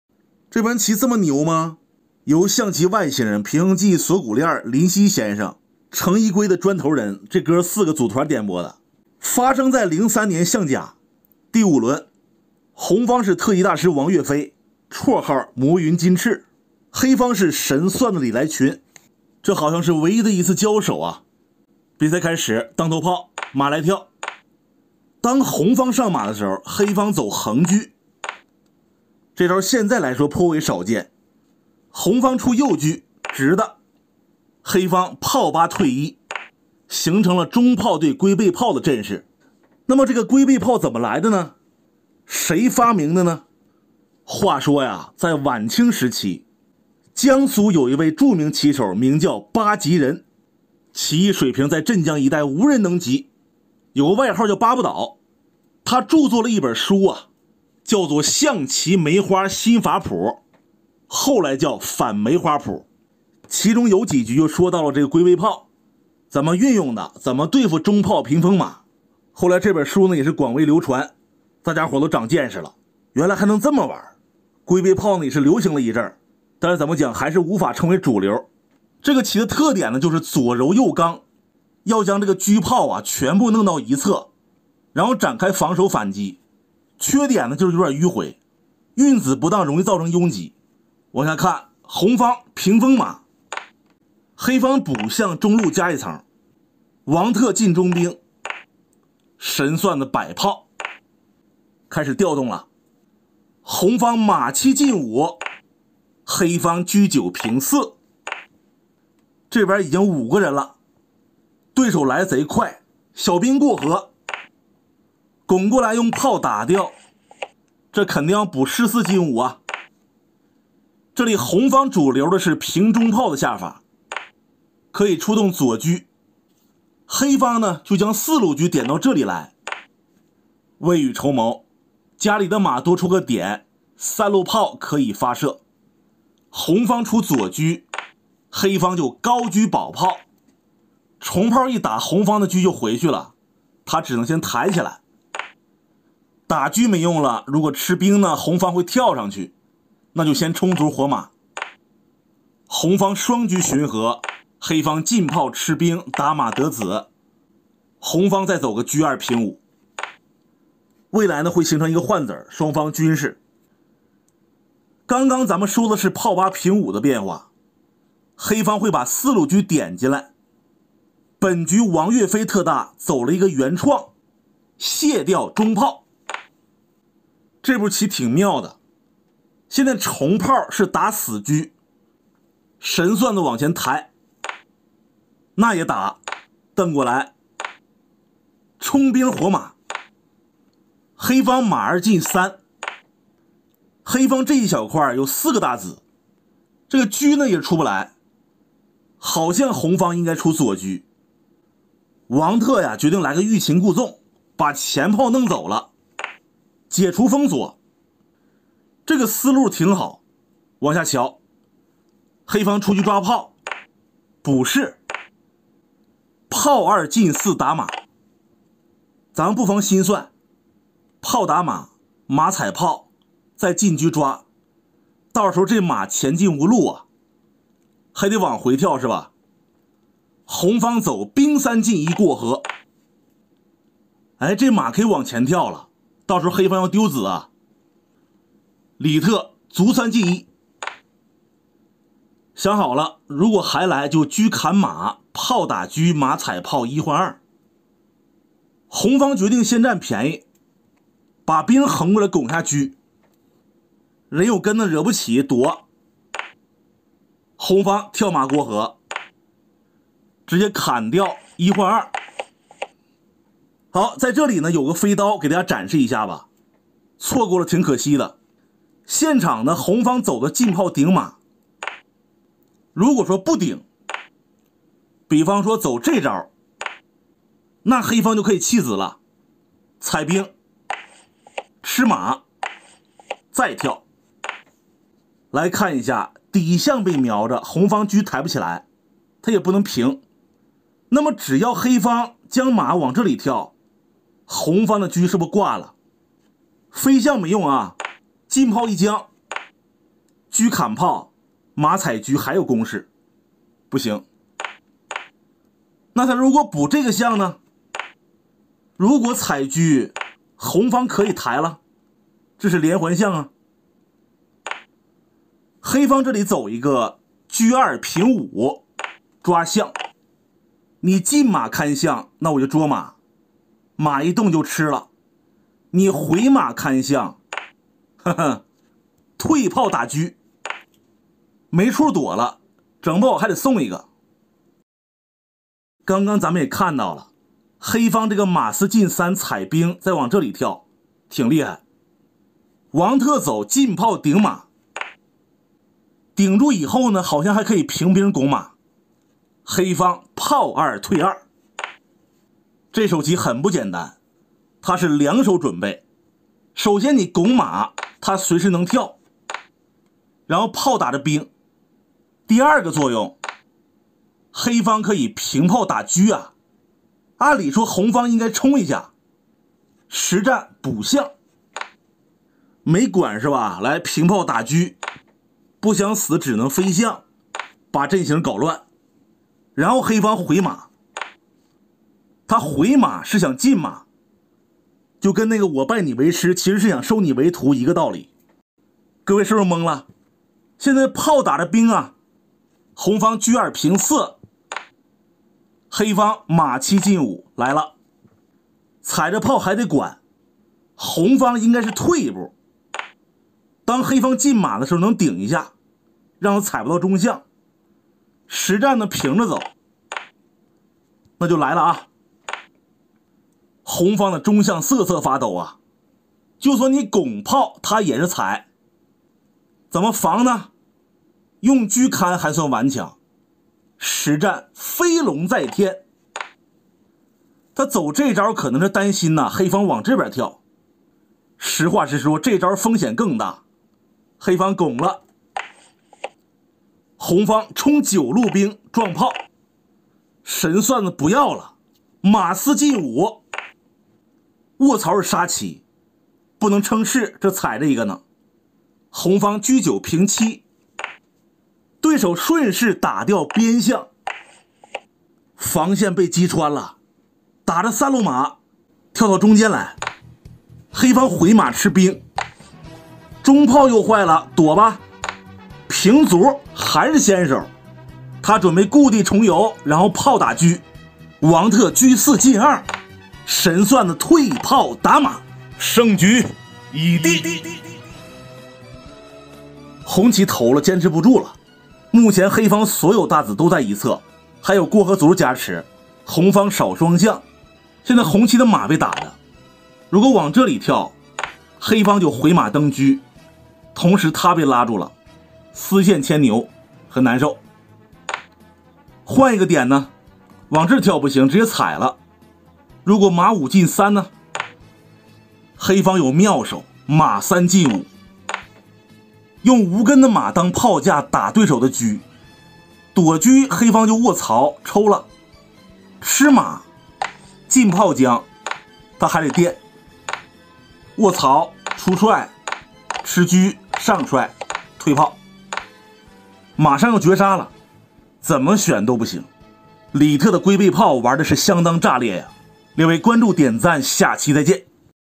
王跃飞vs李来群 无敌龟背炮 火力集于一侧摧枯拉朽【四郎讲棋】
四郎讲棋 象棋 王跃飞 李来群 请注意，本视频完全符合YouTube社区准则，没有任何违规行为。